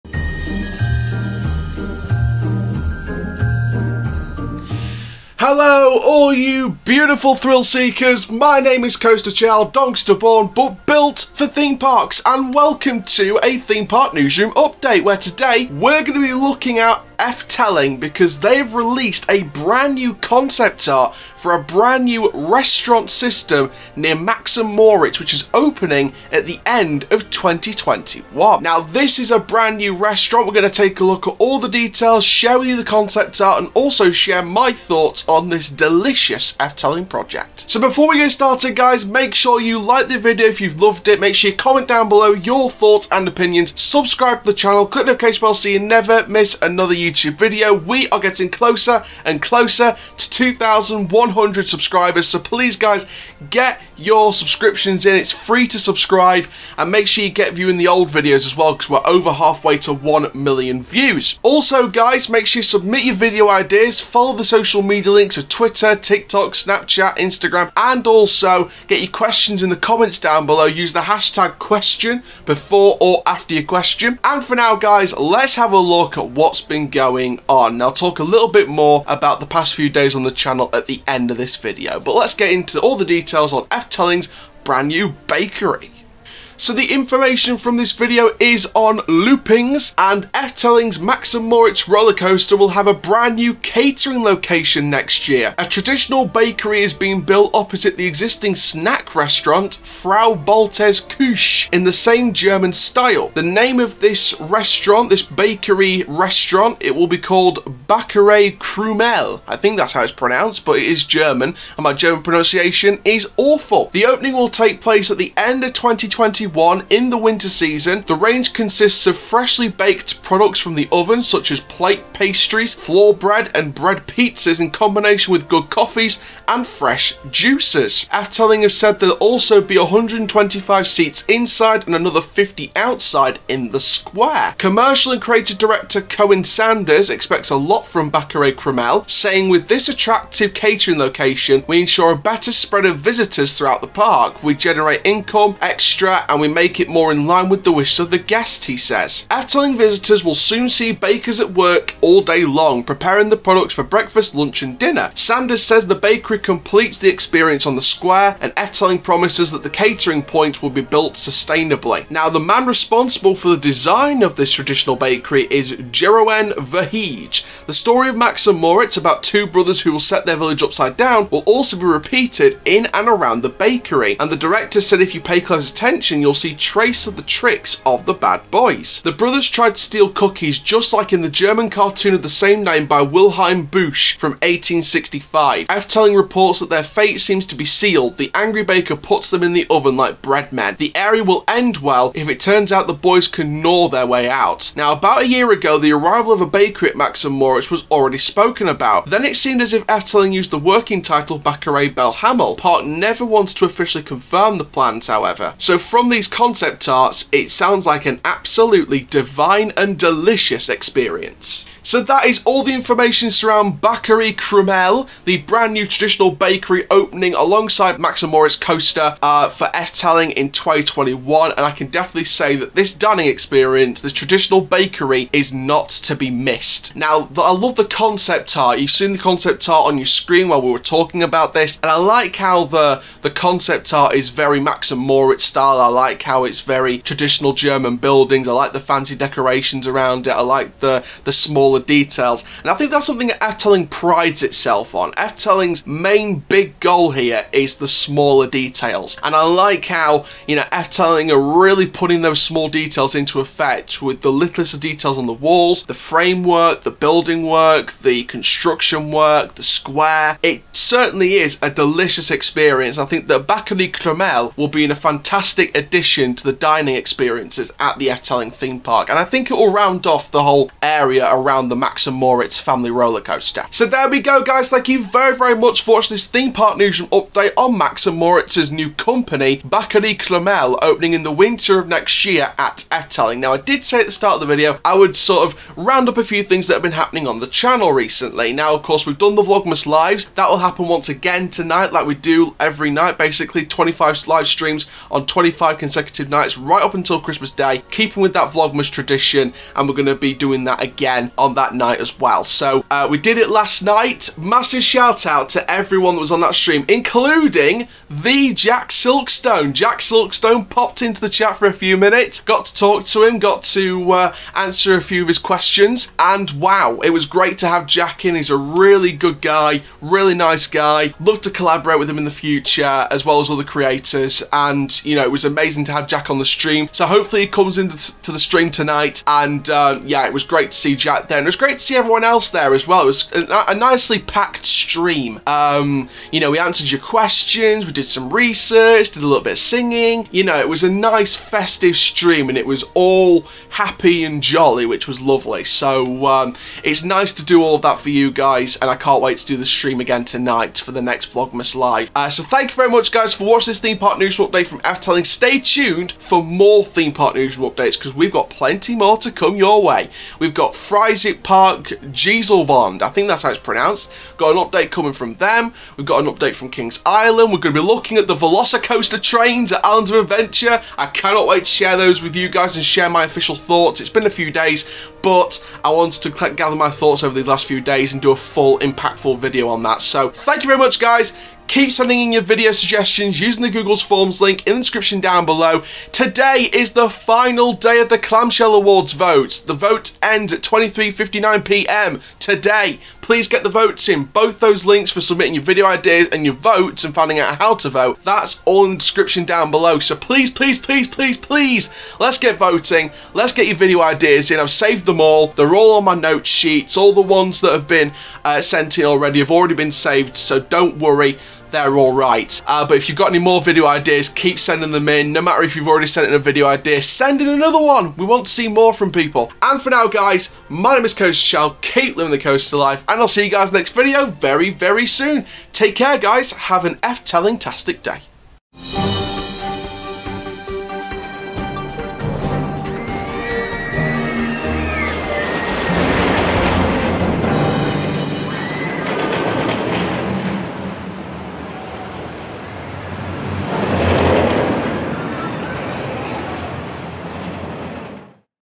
Efteling Has Revealed A New Traditional Bakery Named Backerei Krumel Opening In Winter 2021
On this episode of Theme Park Newsroom, I'll be sharing the details on a new traditional bakery scheduled to open at Efteling towards the end of the 2021 season. I'll also share my thoughts on this delicious looking dining options and discussing why this looks like an amazing addition for the park's dining lineup.

Intro: 0:00
Stats and Facts: 1:26
My Thoughts: 3:32
Outro: 7:08